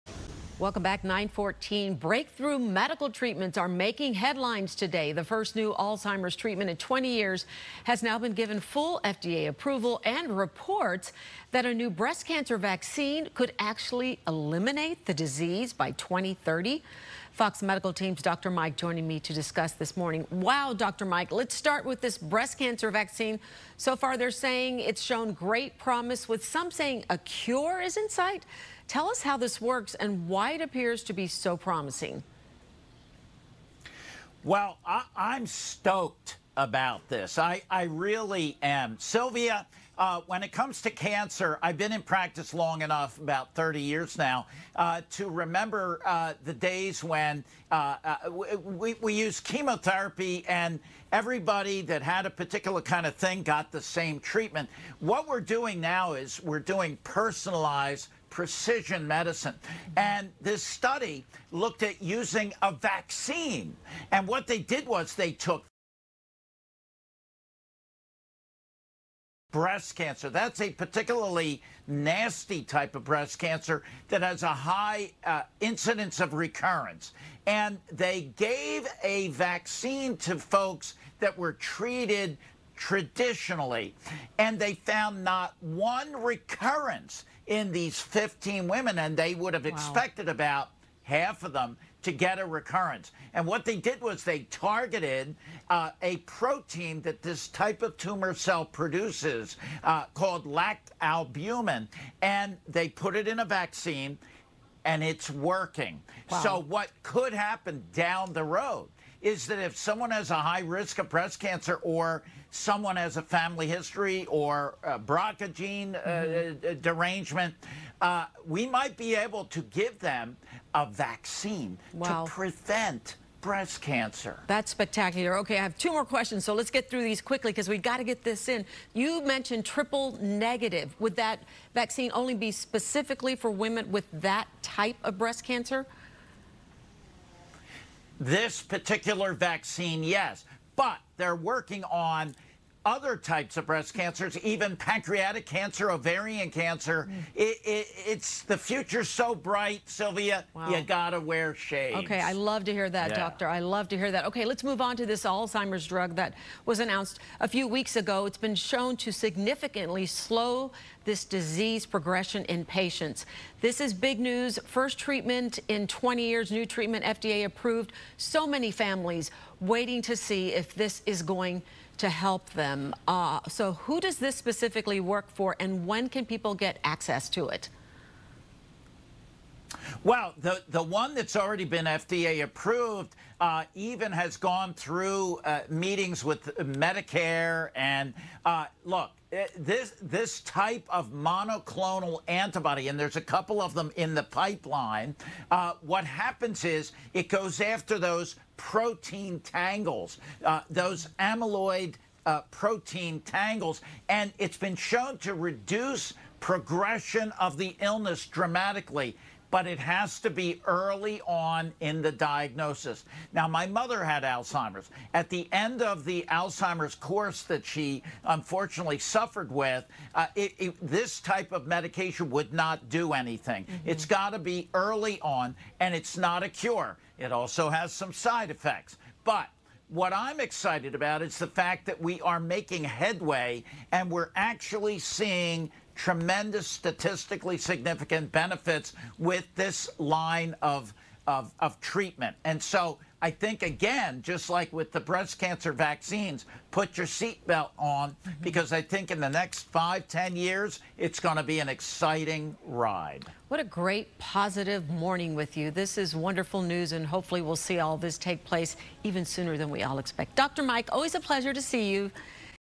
Breast cancer vaccine could eliminate disease by 2030
A breast cancer vaccine is showing great promise, with some saying a cure is in sight.